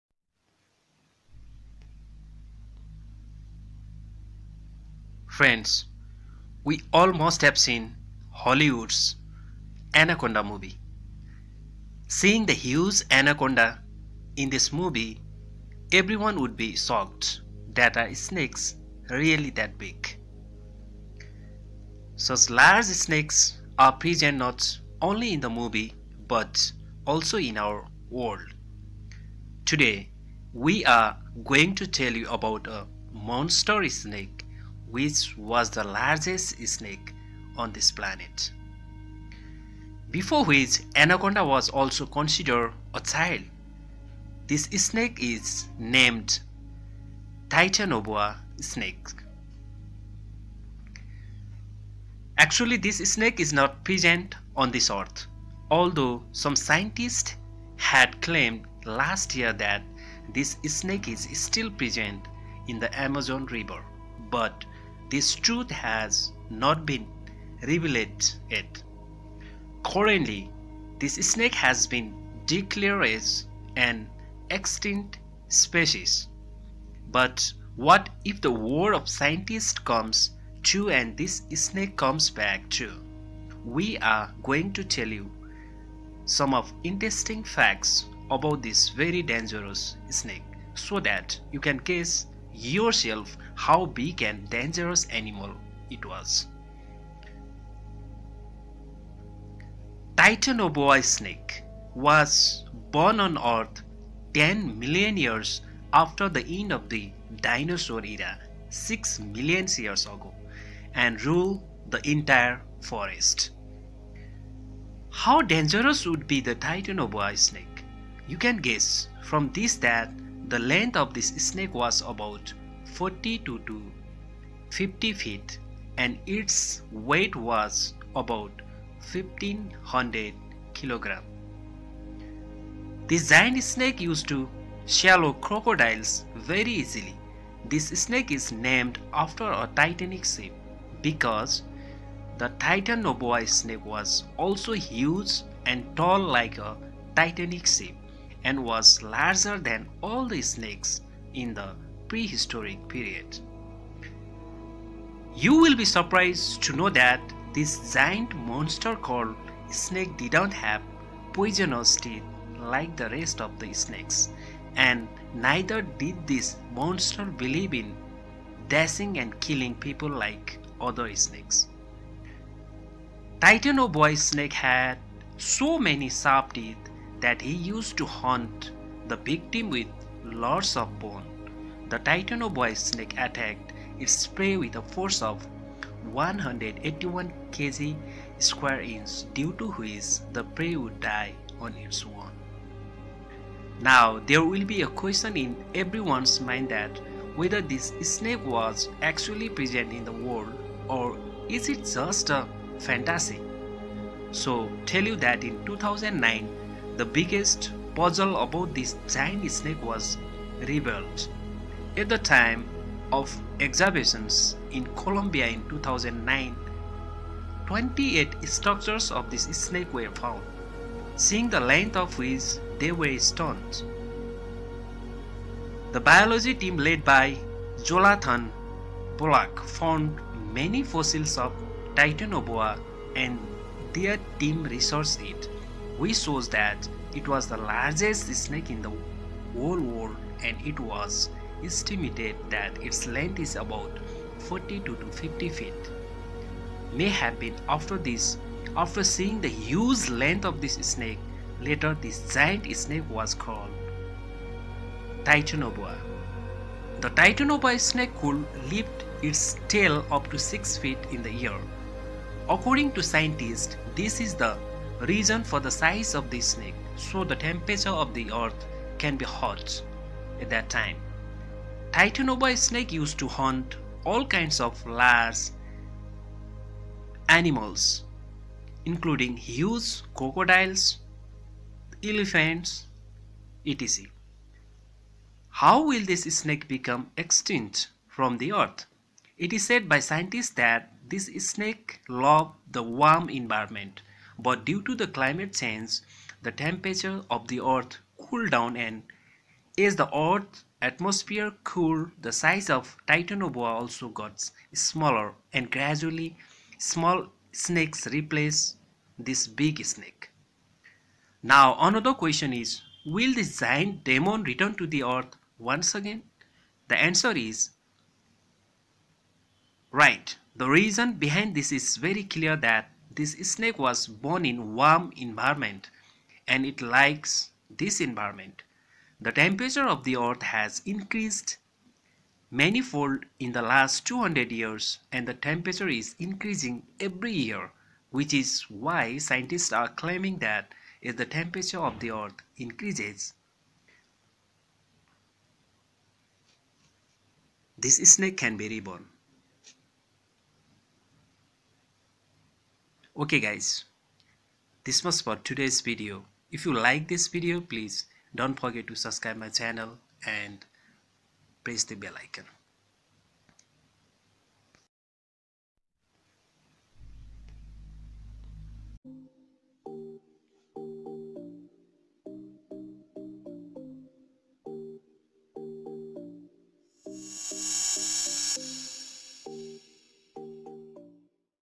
Titanoboa Snake: Five Times Bigger Than Anaconda
History and Introduction About Titanoboa
Thumnail Pic. Source : Hollywood movie
We all must have seen Hollywood's Anaconda movie. Seeing the huge anaconda in this movie, everyone would be shocked that are snakes really that big? Such large snakes are present not only in the movie but also in our world. Today we are going to tell you about a monster snake which was the largest snake on earth, before which anaconda was also considered a child. This snake is named Taitanoboa Snake (Titanoboa Snake).

Which is not present on this earth, although some scientists had claimed last year that this snake is still present in the Amazon River, but this truth has not been revealed yet. Currently, this snake has been declared as an extinct species, but what if the word of scientists comes true and this snake comes back true? We are going to tell you interesting facts about this very dangerous snake, so that you can guess yourself how big and dangerous animal it was.
Taitanoboa Snake (Titanoboa snake) was born on earth ten million years after the end of the dinosaur era six million years ago and ruled the entire forest.
How dangerous would be the Taitanoboa Snake, you can guess from this that the length of this snake was about 42 to 50 feet and its weight was about 1500 kg.
This giant snake used to swallow crocodiles very easily. 4. This snake is named after a Titanic ship because the Taitanoboa snake was also huge and tall like a Titanic ship and was larger than all the snakes in the Prehistoric period.
You will be surprised to know that this giant monster called snake did not have poisonous teeth like the rest of the snakes and neither did this monster believe in dashing and killing people like other snakes.
Titanoboa snake had so many sharp teeth that he used to hunt the victim with lots of bone.
The Titanoboa snake attacked its prey with a force of 181 kg / sq. Inch, due to which the prey would die on its own.
Now there will be a question in everyone's mind that whether this snake was actually present in the world or is it just a fantasy. So tell you that in 2009, the biggest puzzle about this giant snake was revealed. At the time of excavation in Colombia in 2009, 28 structures of this snake were found, seeing the length of which, they were stunned.
The biology team led by Jolathan Baloch found many fossils of Titanoboa and their team researched it, which showed that it was the largest snake in the whole world and it was estimated that its length is about 42 to 50 feet. may have been. After this, after seeing the huge length of this snake, later this giant snake was named "Titanoboa".
The Titanoboa snake could lift its tail up to 6 feet in the air.
According to scientists, this is the reason for the size of the snake, so the temperature of the Earth can be hot at that time.Taitanoboa Snake used to hunt all kinds of large jive up to 10 feet including huge crocodiles. How will this snake become extinct from the earth? It is said by scientists that this snake loved the warm environment but due to the climate change, the temperature of the earth cooled down and as the earth's atmosphere cooled, the size of Taitanoboa also got smaller and gradually Small snakes replaced this big snake.